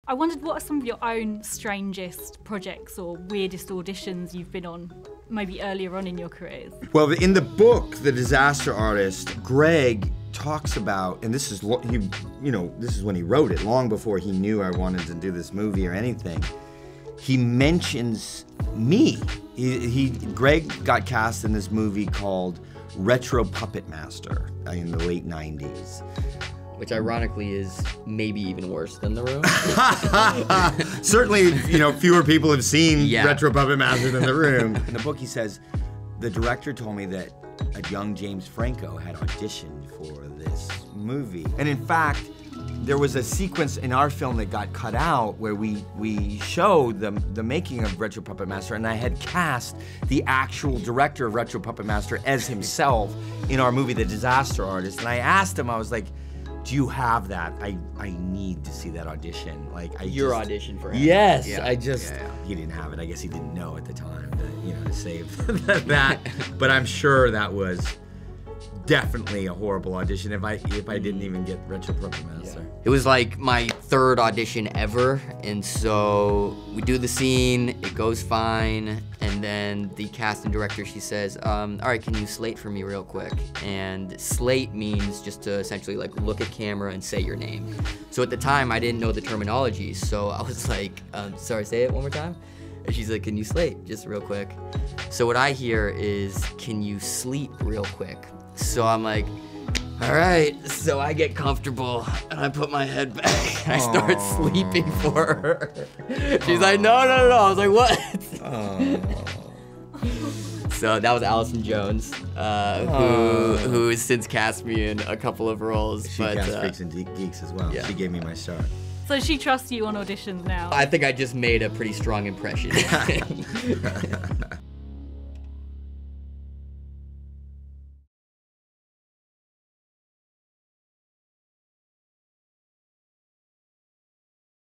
James & Dave Franco Reveal Their CRAZIEST Auditions Ever | MTV Movies
The stars of new movie The Disaster Artist reveal their IRL strangest movie auditions. And you thought The Room was weird…